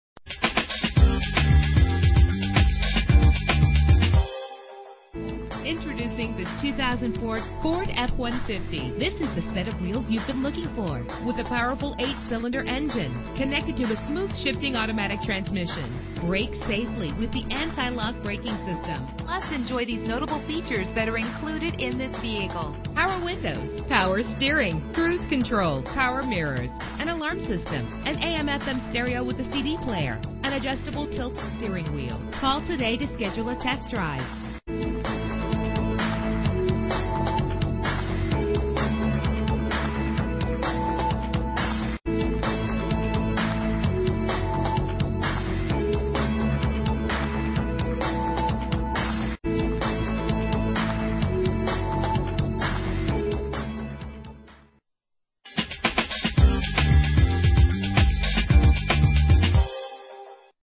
2004 Ford F-150 - Wayne NJ
Year: 2004
Make: Ford
Model: F-150
Trim: XLT
Engine: 5.4 liter 8 cylinder 24 valve
Transmission: 4-Speed Automatic
Color: Silver Metallic
Mileage: 58281
Address:
444 Route 46